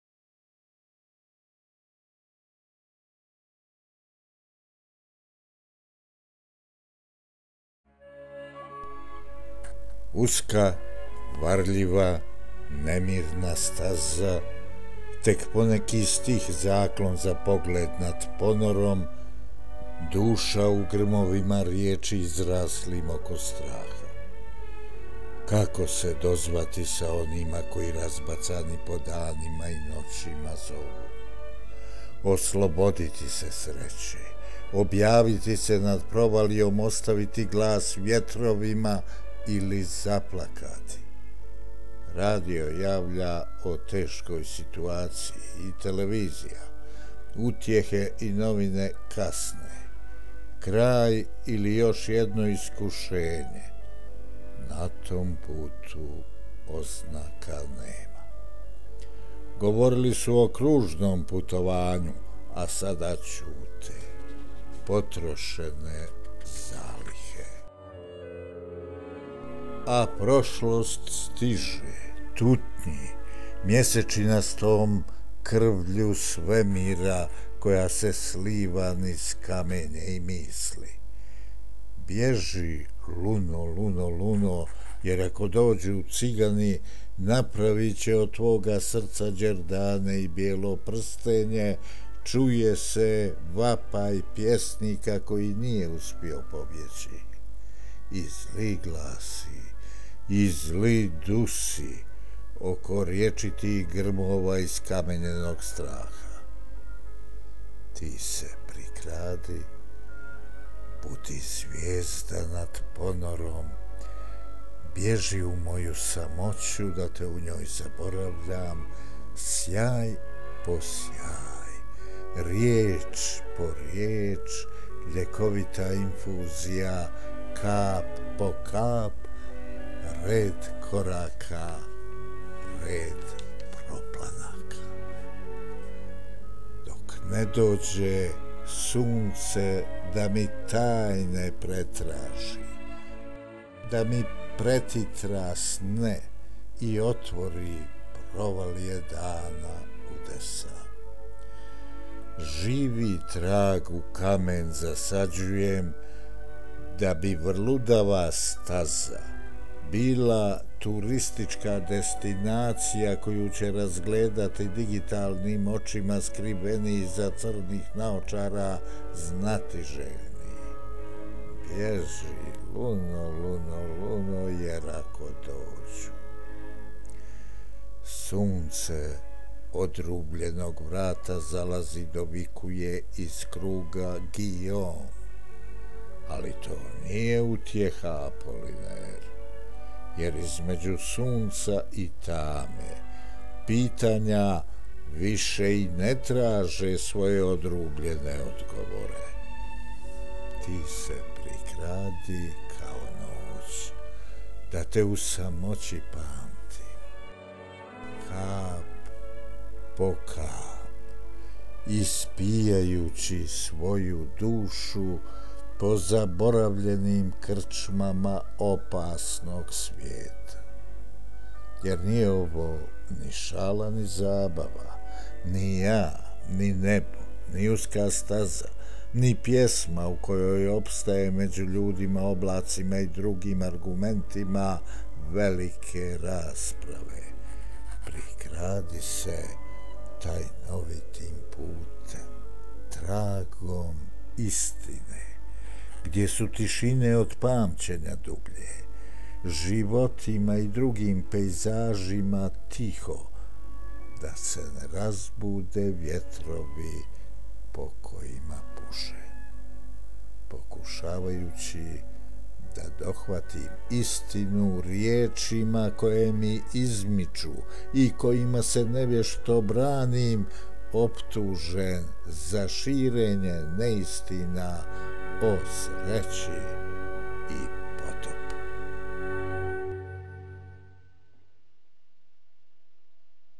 Pavle STANIŠIĆ - Zvijezda nad ponorom
Književnik P.Stanišić kazuje vlastite stihove